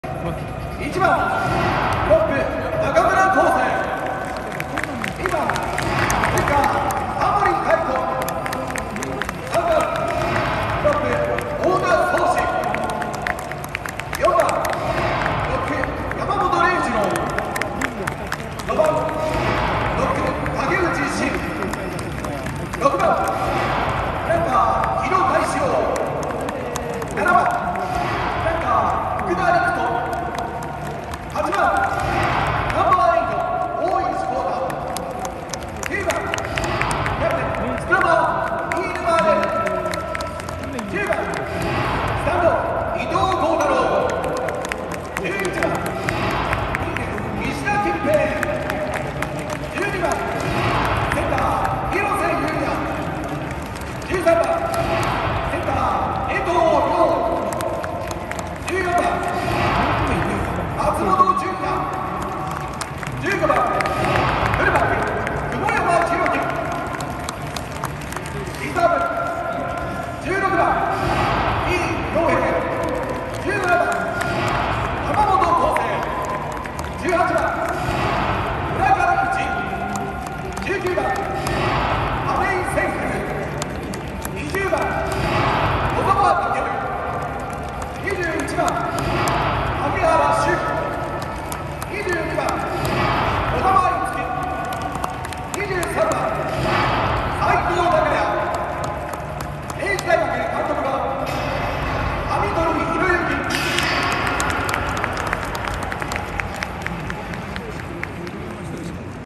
明治大 選手紹介 第58回ラグビー全国大学選手権決勝 帝京vs明治
第58回全国大学ラグビーフットボール選手権大会 決勝
2022/01/09 (日) 13:15 kickoff 国立競技場
観客数　24,275人
レフリー梶原晃久
帝京　27 - 14 明治

帝京大学の四年ぶり10度目の日本一

帝京大学
スターティングメンバー
FW
1 照内寿明
2 江良颯
3 細木康太郎
4 本橋拓馬
5 江里口真弘
6 青木恵斗
7 上山黎哉
8 奥井章仁
HB
9 李錦寿
10 高本幹也
TB
11 二村莞司
12 押川敦治
13 志和池豊馬
14 白國亮大
15 谷中樹平

リザーブメンバー
16 小林恵太
17 津村大志
18 奥野翔太
19 山川一瑳
20 リッチモンド・トンガタマ
21 岡本泰斉
22 松山千大
23 ミティエリ・ツイナカウヴァドラ

明治大学
FW
1 中村公星
2 田森海音
3 大賀宗志
4 山本嶺二郎
5 武内慎
6 木戸大士郎
7 福田陸人
8 大石康太
HB
9 飯沼蓮
10 伊藤耕太郎
TB
11 石田吉平
12 廣瀬雄也
13 江藤良
14 松本純弥
15 雲山弘貴

16 紀伊遼平
17 山本耕生
18 村上慎
19 亀井茜風
20 小澤翔
21 萩原周
22 児玉樹
23 齊藤誉哉